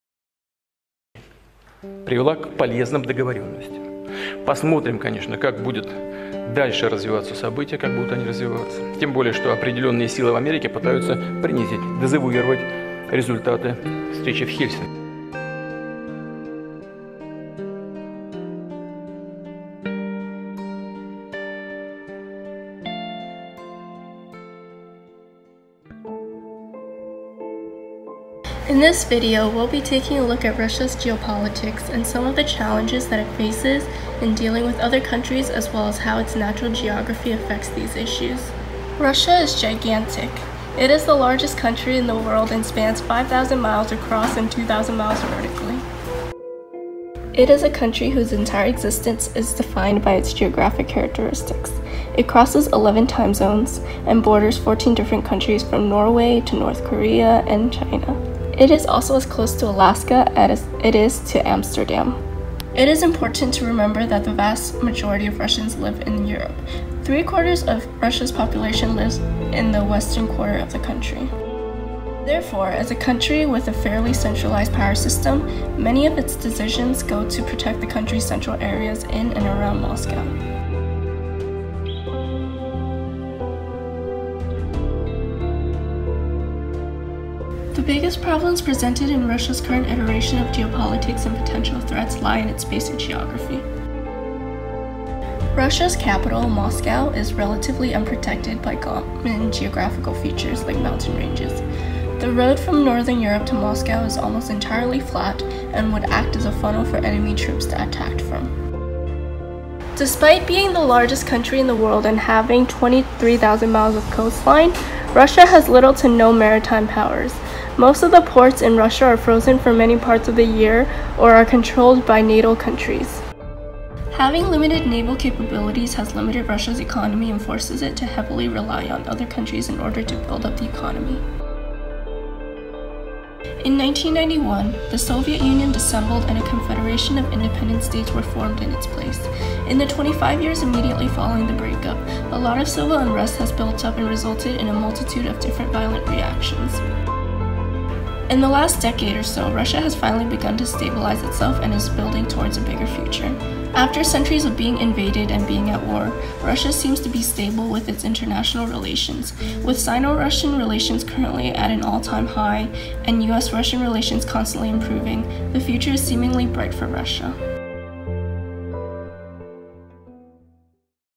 DEFINING RUSSIA: The Geopolitics of the World’s Largest country
Russian geopolitics due to geography and the effect of other countries. Factors that come into play are mainly due to its geography.

-German, Tracey. "Russia, Eurasia and the New Geopolitics of Energy: Confrontation and Consolidation. Edited by Matthew Sussex and Roger E. Kanet." International Affairs 92, no. 5 (2016): 1278-279.

-Kotkin, Stephen. "Russia's Perpetual Geopolitics: Putin Returns to the Historical Pattern." Foreign Affairs 95, no. 3 (2016): 2-9.

-Makarychev, Andrey, and Alexandra Yatsyk. "Biopower and Geopolitics as Russia’s Neighborhood Strategies: Reconnecting People or Reaggregating Lands?" Nationalities Papers 45, no. 1 (2017): 25-40.

-Miller, Chris. The Struggle to save the Soviet Economy: Mikhail Gorbachev and the Collapse of the USSR. New Cold War History. Chapel Hill: University of North Carolina Press, 2016.